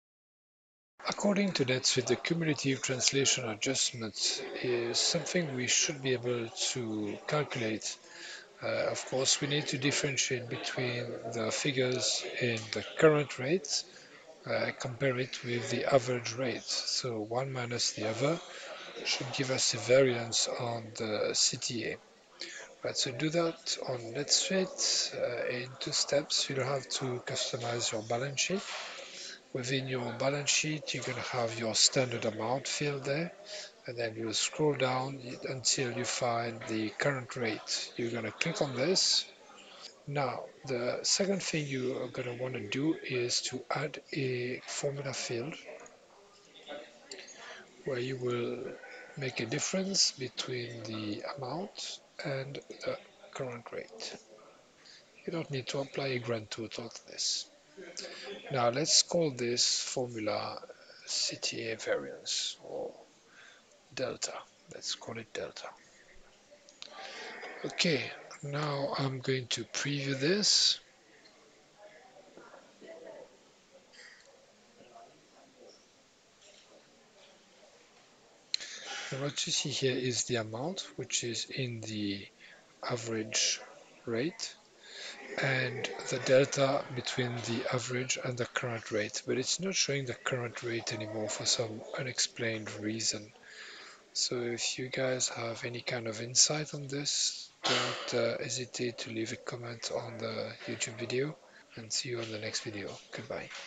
CTA cumulative translation adjustment on netsuite oneworld
The mysterious case of the disappearing 'Amount (Current rate)' from the Balance Sheet.